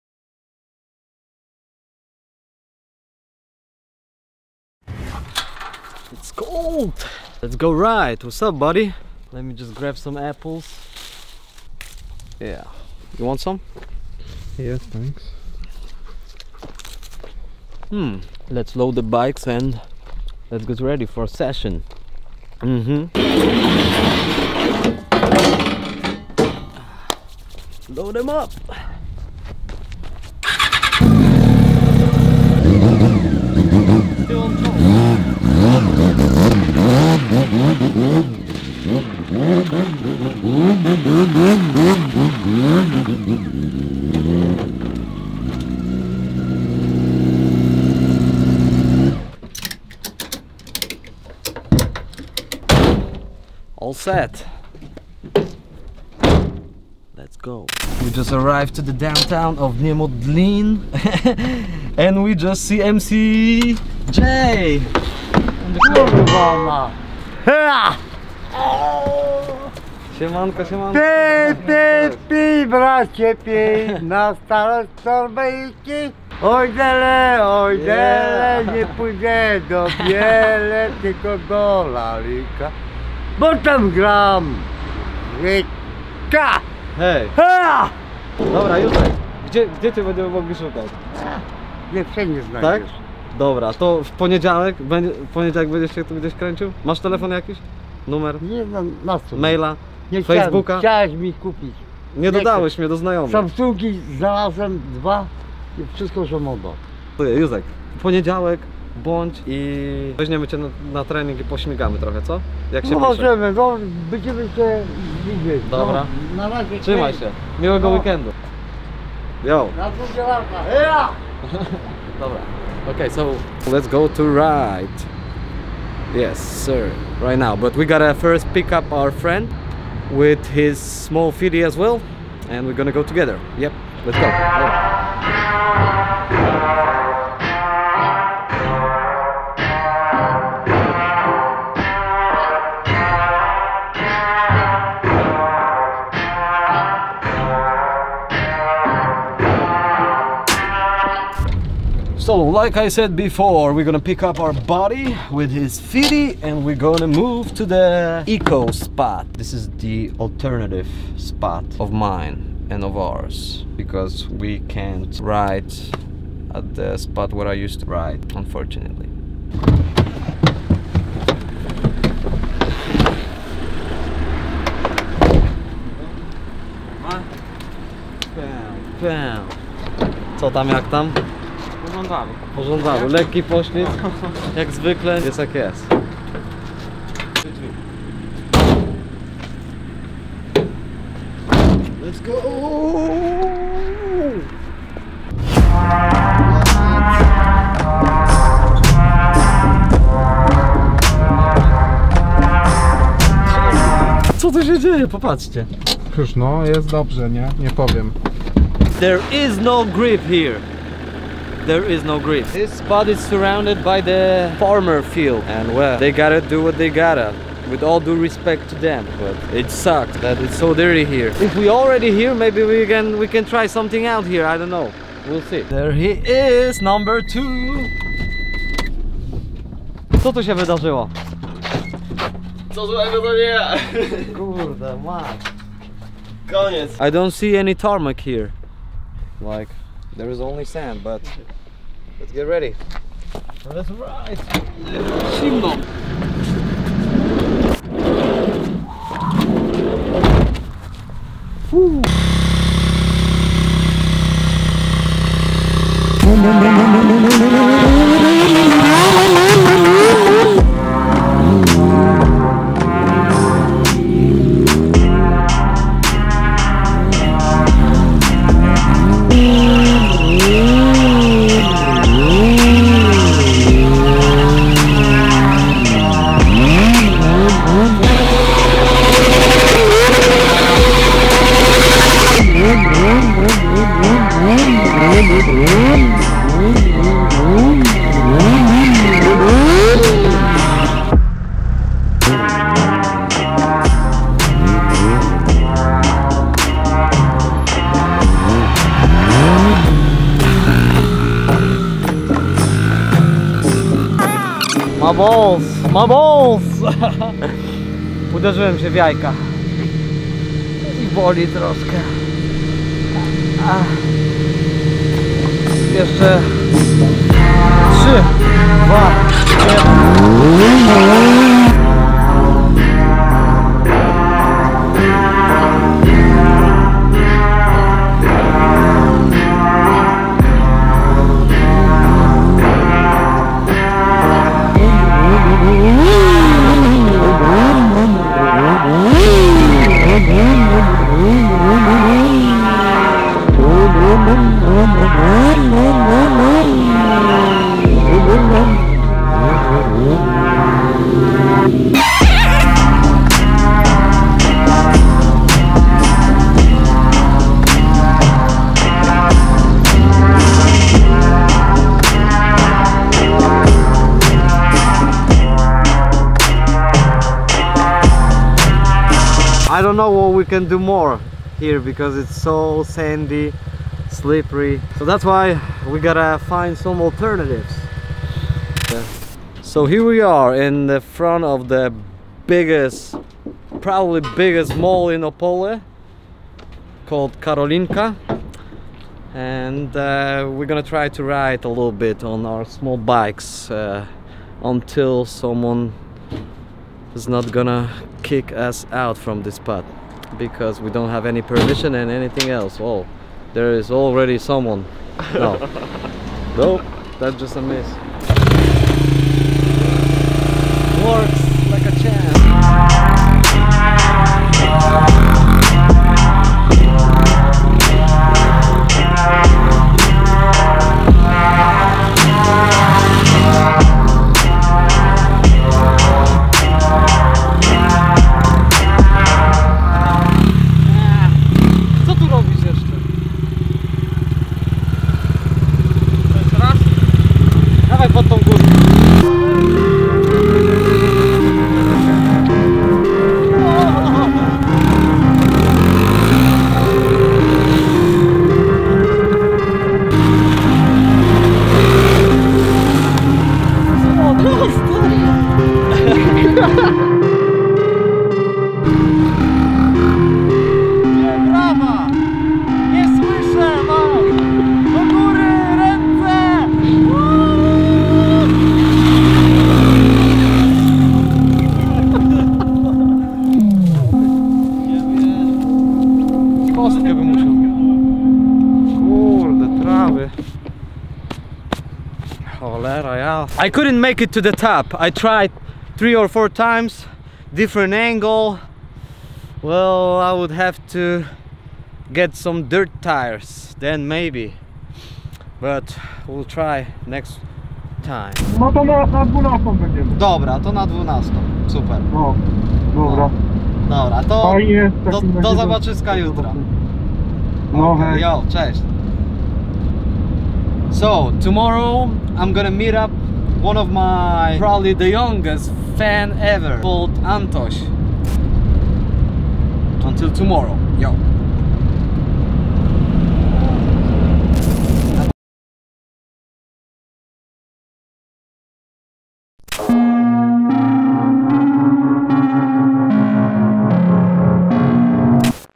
WE MET MC JOZEF AND COULDN'T FIND A PROPER PLACE TO RIDE
Once upon a time in Novembaaa... we were struggling to find any proper place to ride , but somehow we met our old friend MC JOZEF ! which is as hard as sending the crew to Mars...